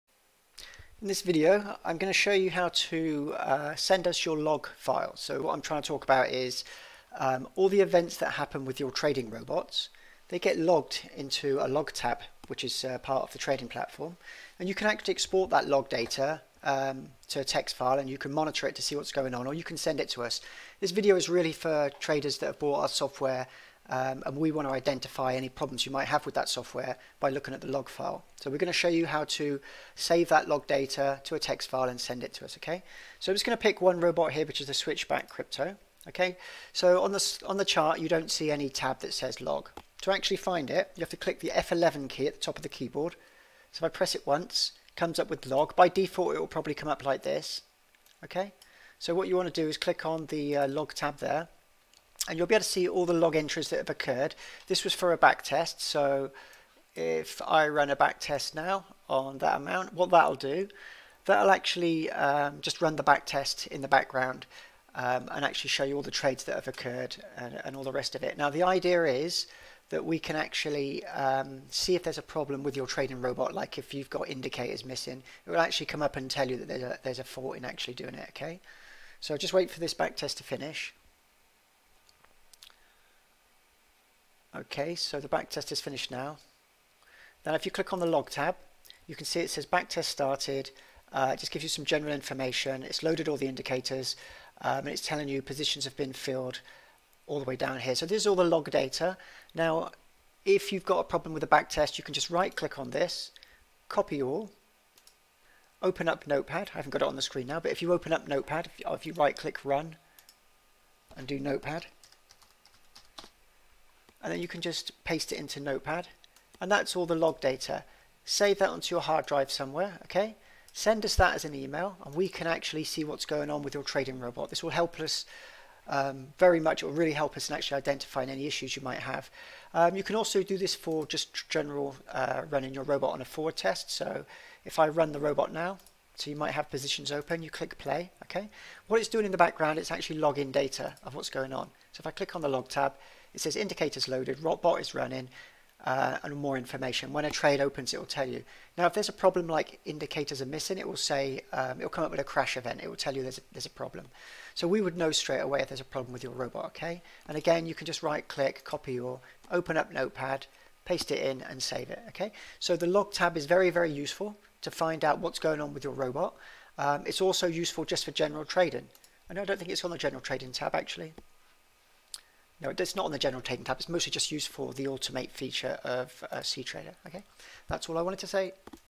cTrader - How To Export Historical Trade Information
This video will show you how to export trade history as a log file and all trade information for automated trading systems, this will help both us and you identify any issues you may have with your system.